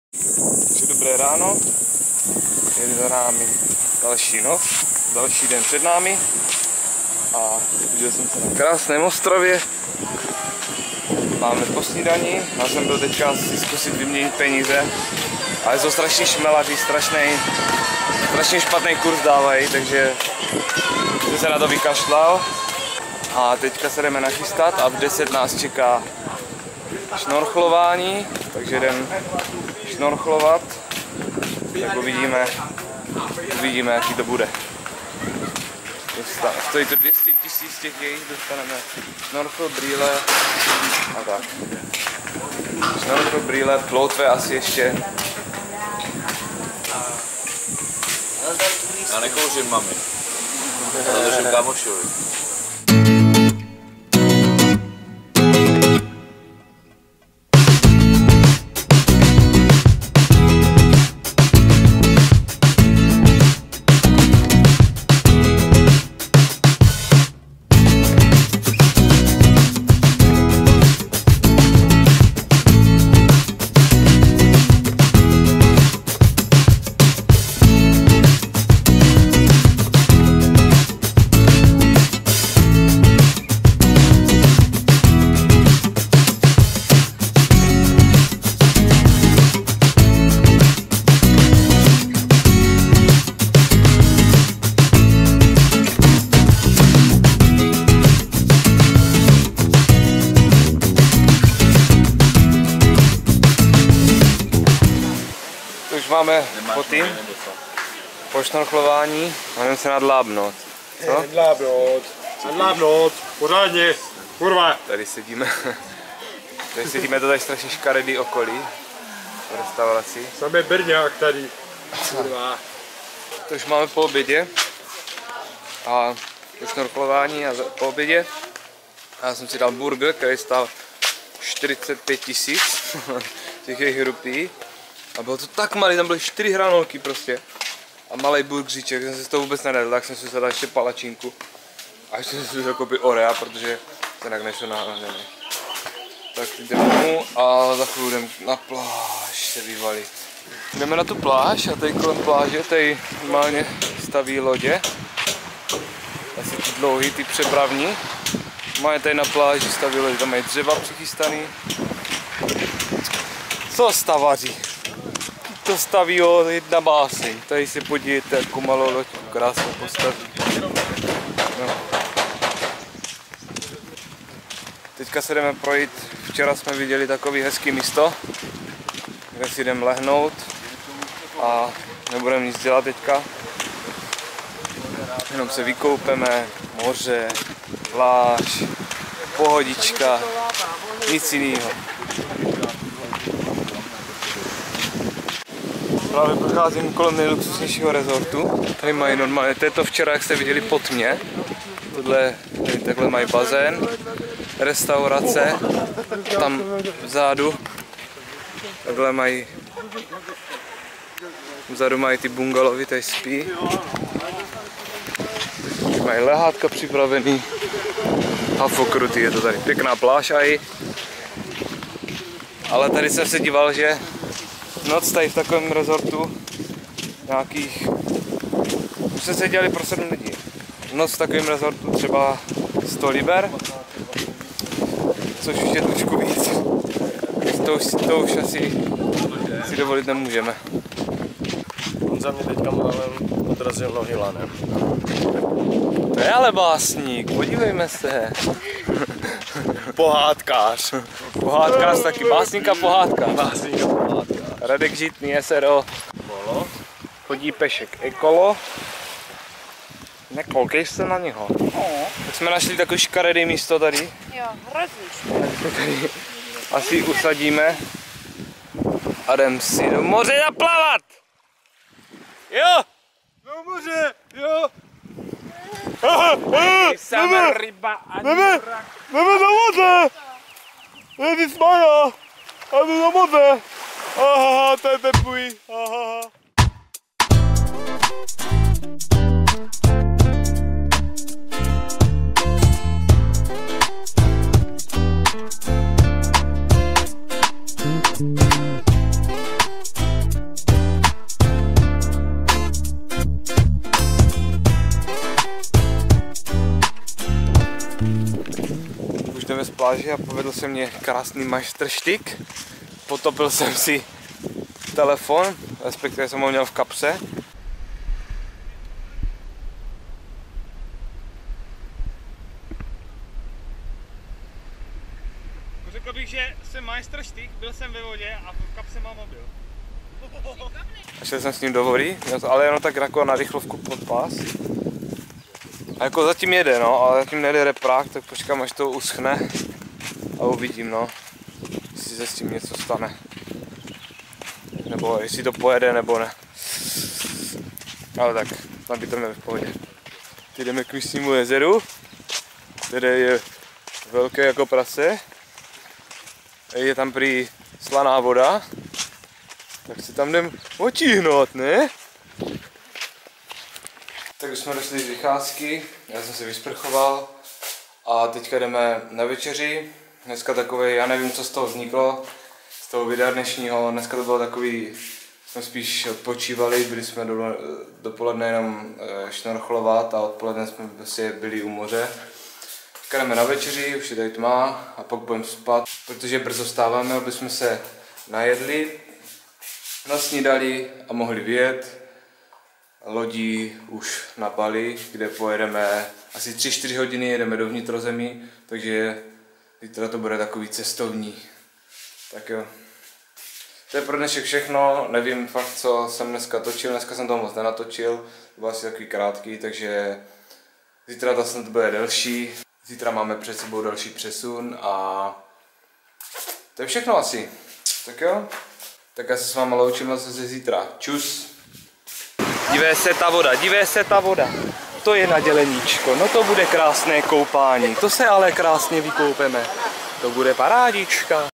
Utopenej iPhone l Asie Trip Den 7
Tento den byl odpočinkovej to sem toho tolik nenatočil ale za to se mi povedlo utopit telefon. Jel ale staral sem se v nabijecim portu kterej sem pokazil a musel po příjezdu vyměnit.